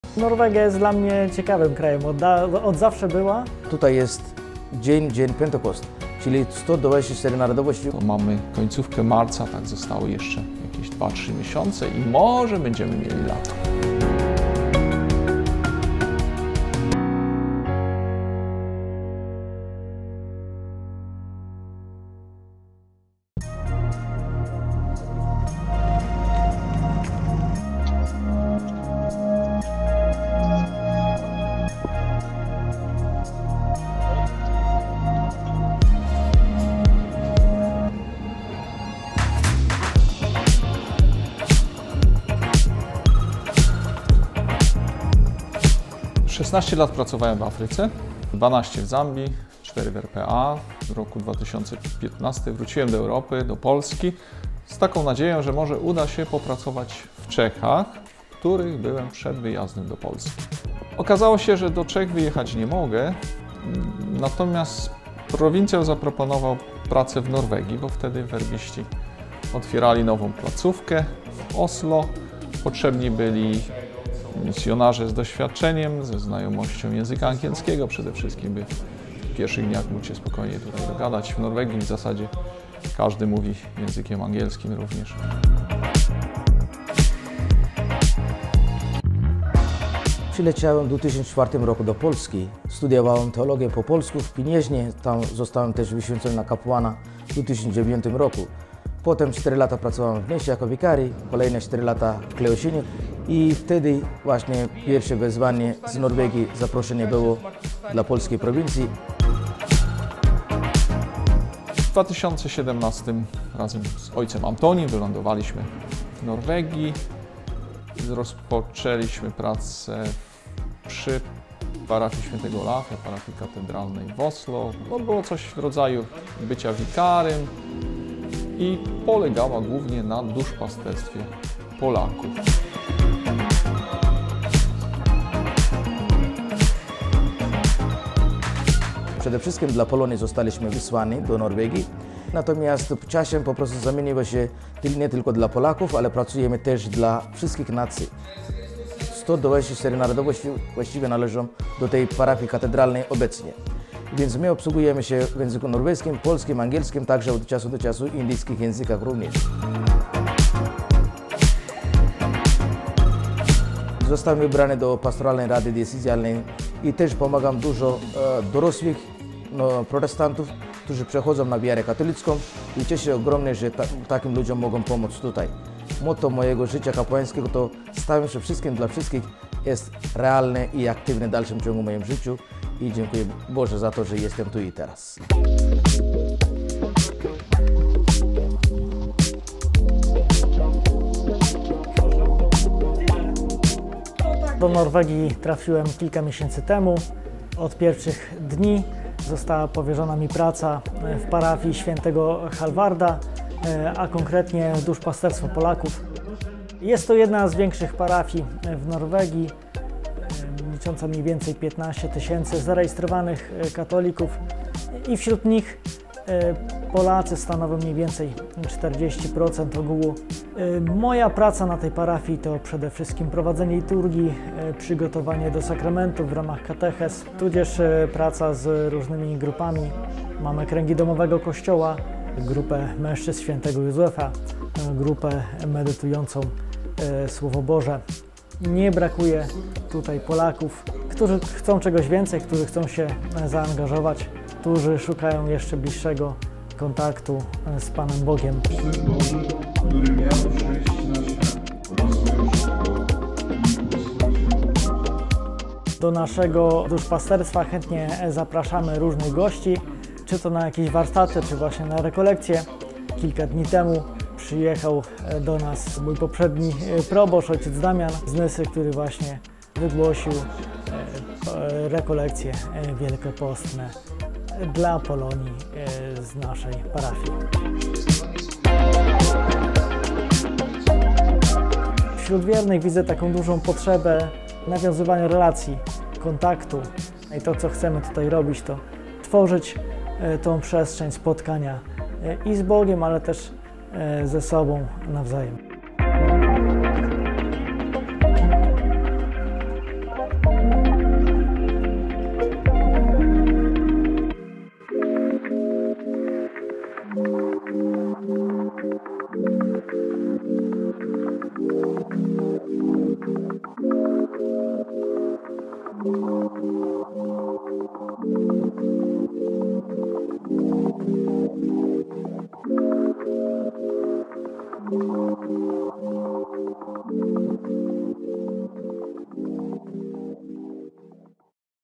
Werbiści i migranci w wielkim mieście - OSLO
Norwegia liczy 5,3 mln mieszkańców. Imigranci stanowią 18% ogółu ludności tego kraju. Jedna trzecia ludności Oslo, stolicy Norwegii, posiada imigranckie korzenie. Największą grupą imigrantów w Norwegii są Polacy. Jest ich około 120 tys. Dzisiaj wśród polskich emigrantów w Oslo pracuje trzech polskich werbistów.